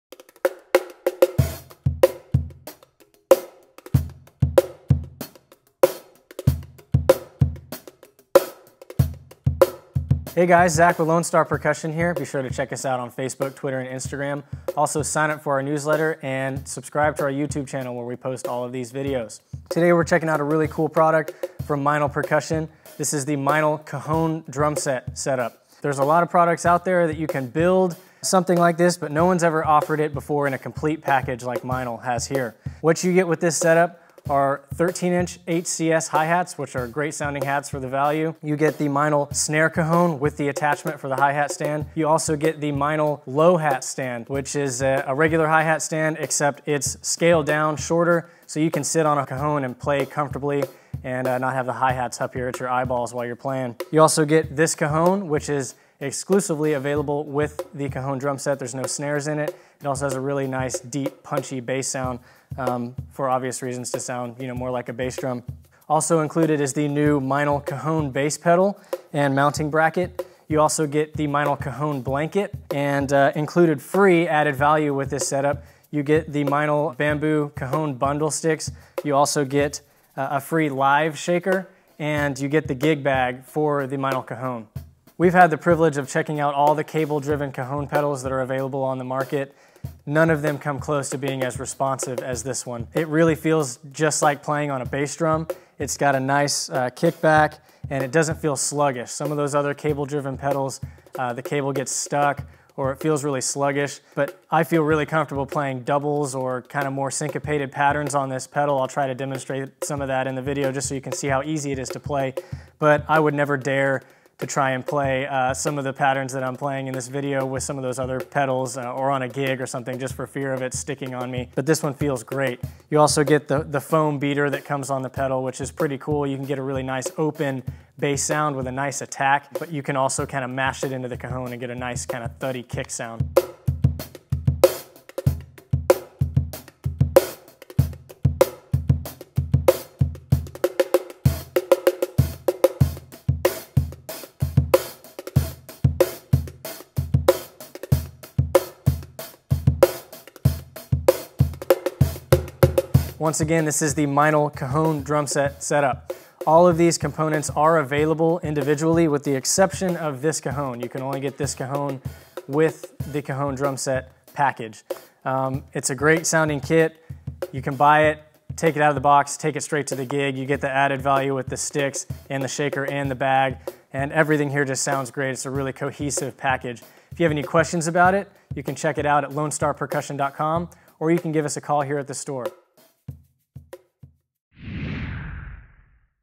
Meinl Drum Set Cajon
Replacing the traditional drum set with a "cajon drum set" is becoming very popular, especially for you drummers who need something a little smaller, quieter, and more exotic. Meinl has made it easier than ever to get everything you need with this Cajon Drum Set Package!

This package includes absolutely everything - the instruments, the hardware, and some other goodies:

SCAJ100LB-NT Headliner Series Cajon (No Snares)
TMCP Cajon Pedal
SW-HH Hi Hat Stand
HCS13H 13" HCS Hi Hat Cymbals
MCS1-BK Mountable Cajon Snare
HMC-1 Standard Multi-Clamp
CAJ-BLK Cajon Blanket
FREE MSTCJB Cajon Bag
FREE BCMS1 Bamboo Cajon Multi Sticks
FREE SH4BK Luis Conte Shaker
The Cajon Drum Set gives you a ton of options when it comes to playing. Use the foot pedal or your hands to give you a bass tone, and your hands or the multi-sticks on the Snare Cajon for a backbeat! Add in some hi hat action with the sticks, or grab the shaker - it's really all up to your coordination.